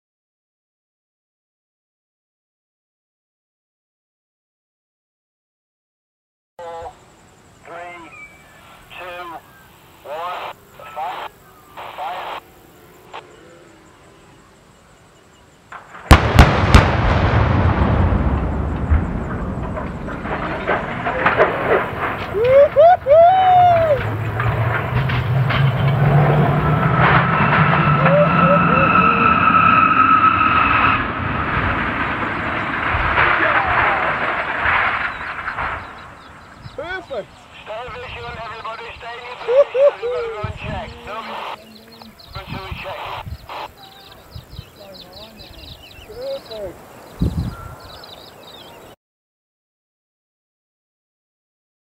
Omega Navigational Mast Demolition and Removal
Liberty Industrial are currently undertaking the demolition and
removal of the Omega Navigational Mast in Darriman, Victoria for the Department of Defence. At 432 metres high, the steel tower was the tallest structure in the Southern Hemisphere. Explosive demolition methods were employed to bring the redundant Naval communications tower to ground level.